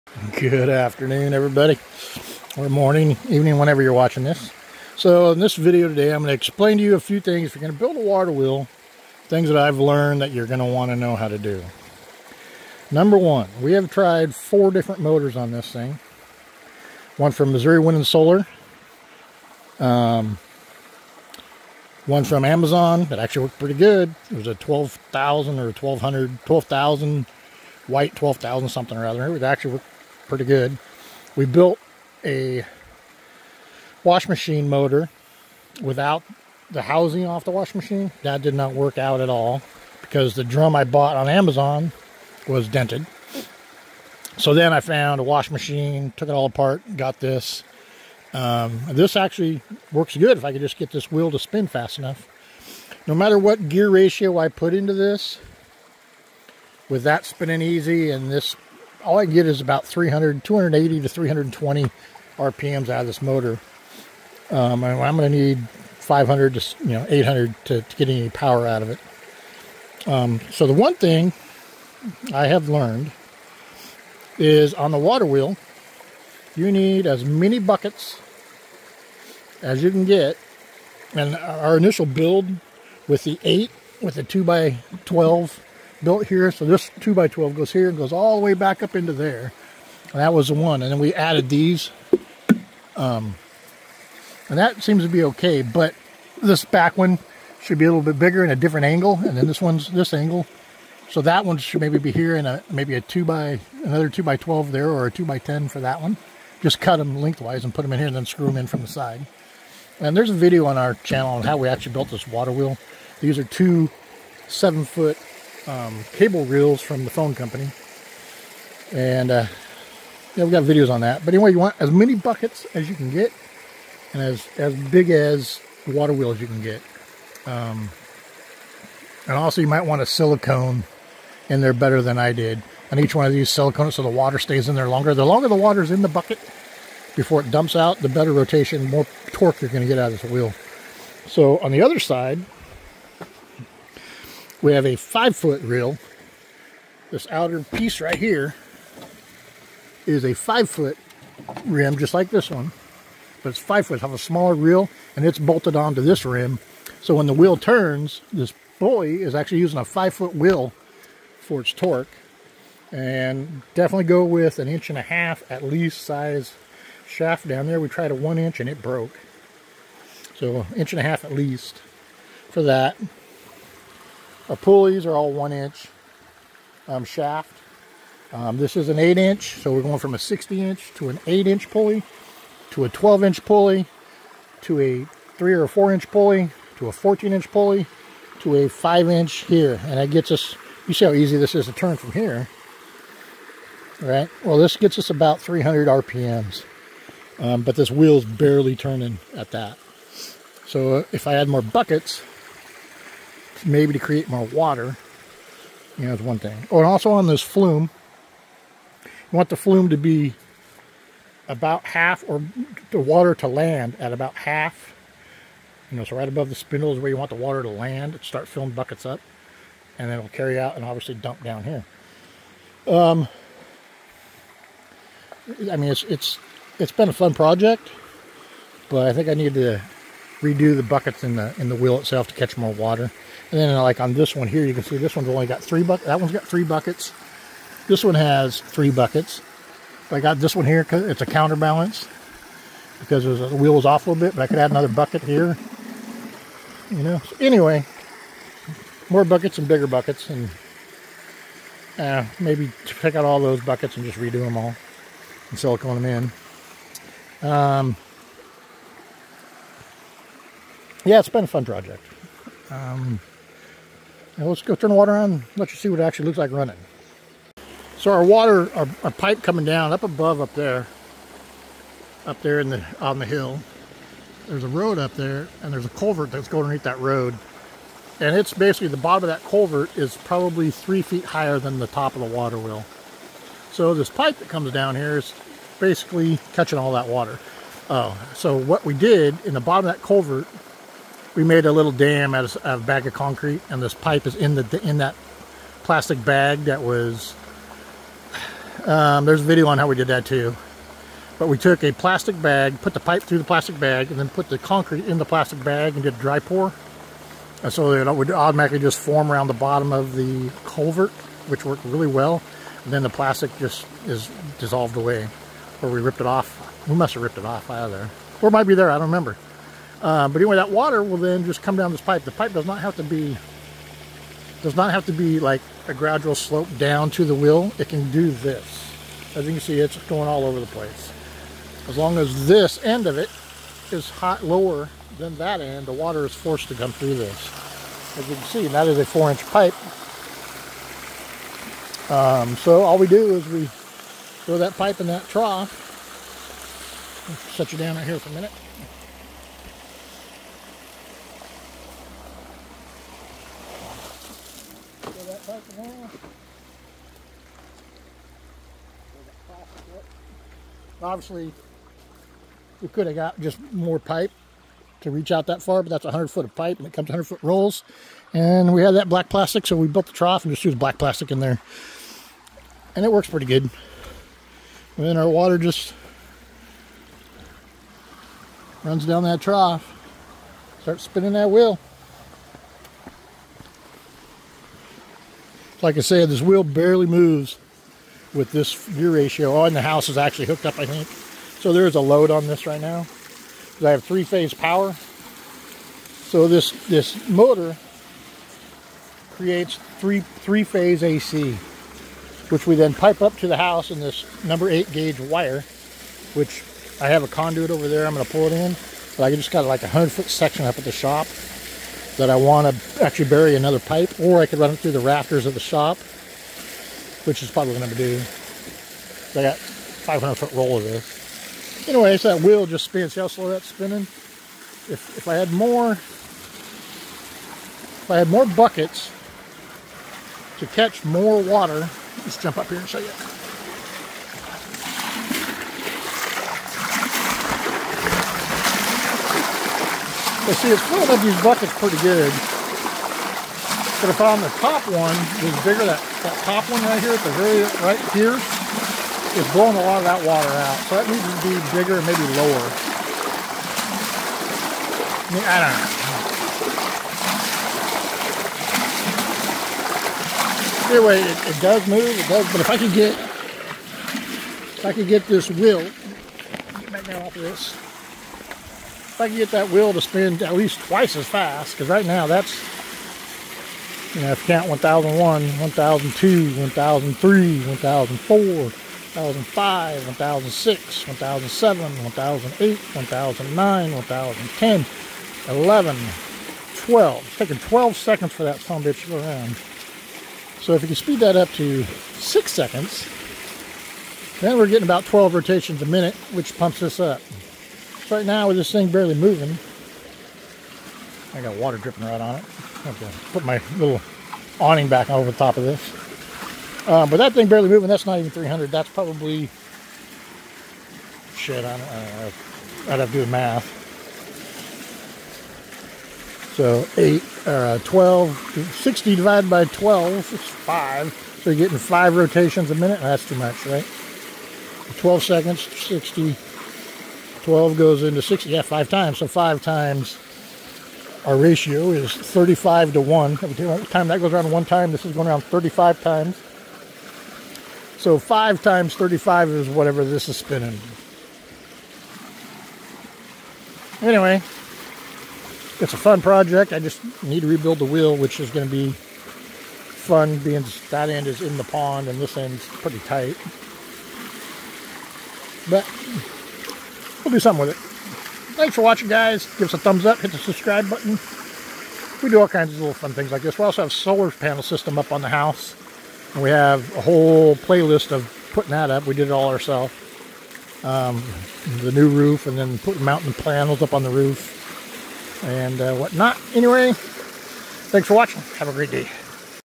How to Build a Waterwheel Generator, Here is my best advice.
Can a waterwheel generate free power to help charge a battery bank that is hooked upto a Solar power system?

In this video I go over the best advise I can give on building a waterwheel generator.

I show you the most important parts of building the waterwheel and even gear ratio vs waterwheel torque.

In this video we have a washing machine stator motor hooked up and the load from hooking it up to the solar system slows it down to about 100 rpm at the washing machine motor.

The smaller Amazon China build PMG actually does better then the washing machine motor when hooked up to our waterwheel.

Our water wheel is made out of 7 foot cable reels and lumber.

0:00 Intro
0:14 PMG motors
0:53 Gear ratio
1:14 Waterwheel buckets
1:53 Waterwheel structure
2:25 Gear ratio again
3:30 Water wheel water flume
4:28 Turning on the water
6:35 3 phase power to house
7:16 waterwheel buckets up close
7:55 Gear ratio with water running